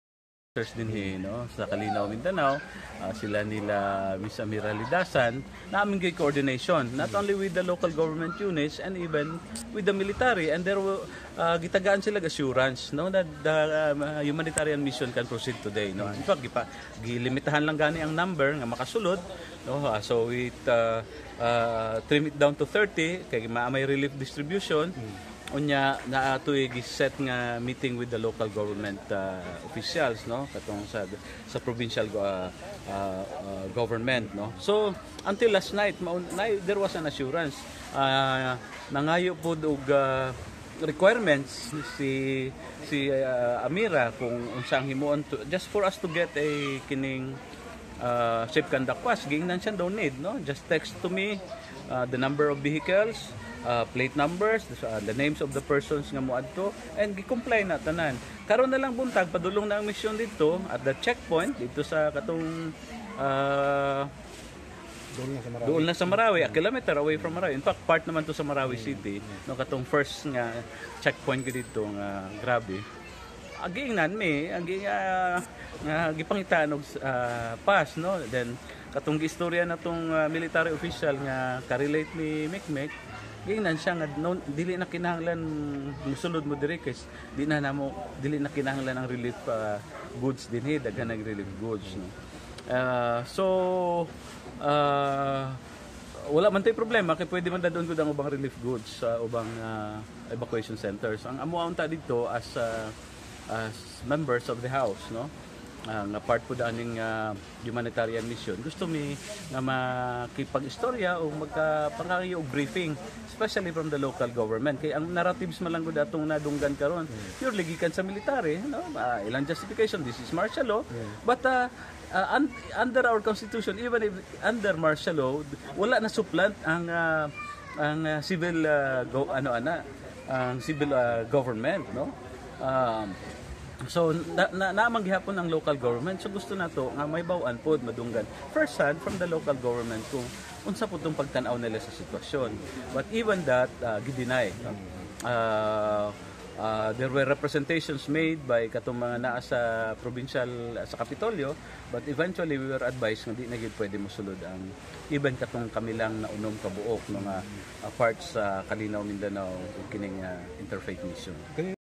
Bayan Muna Rep. Carlos Zarate's reaction after they were denied entry to Marawi City
A team from the National Interfaith Humanitarian Mission were denied entry by the military to Marawi City on Thursday, June 15.